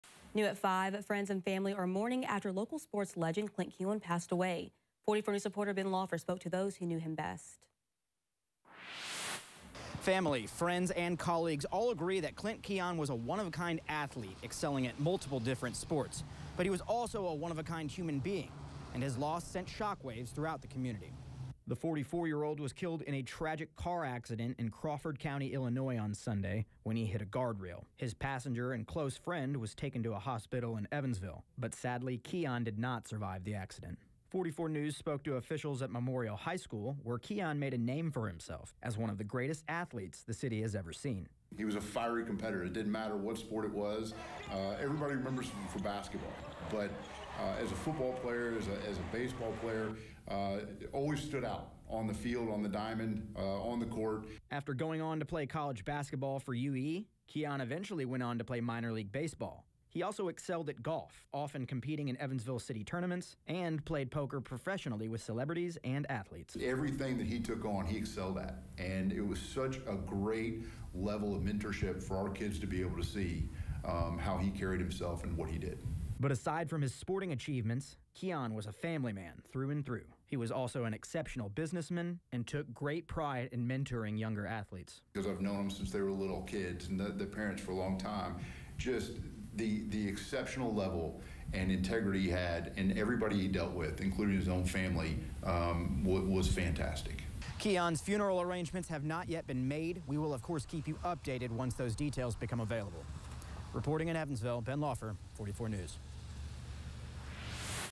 Friends and colleagues remember Evansville sports legend Clint Keown
The former Memorial High School standout's family announced his passing on Sunday.